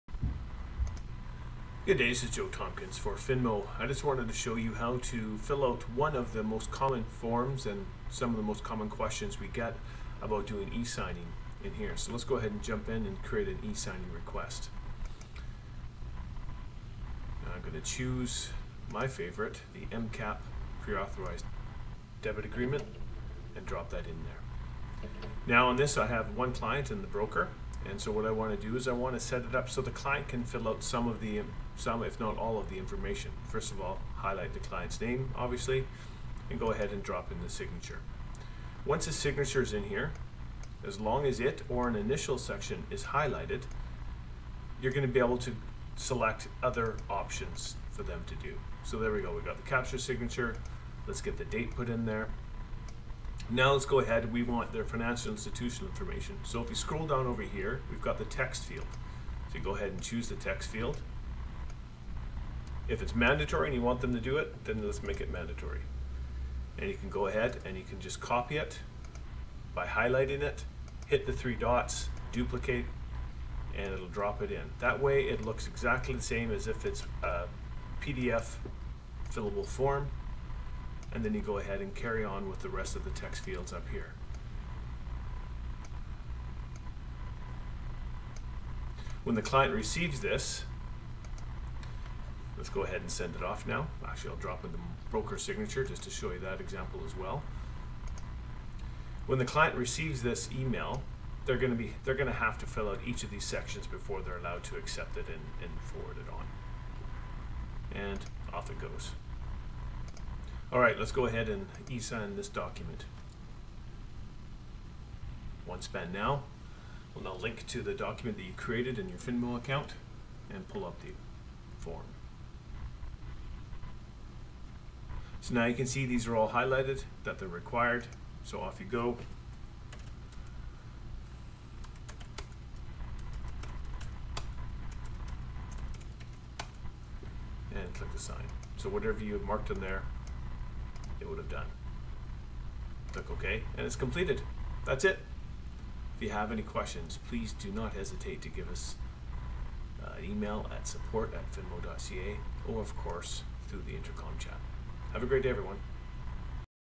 Finmo - How to prep a PAD form for E-Signature
A common troublesome E-signing form, the PAD. This video shows you how to use the full functionality of E-signing in Finmo to deliver a 'paperless' experience to your clients.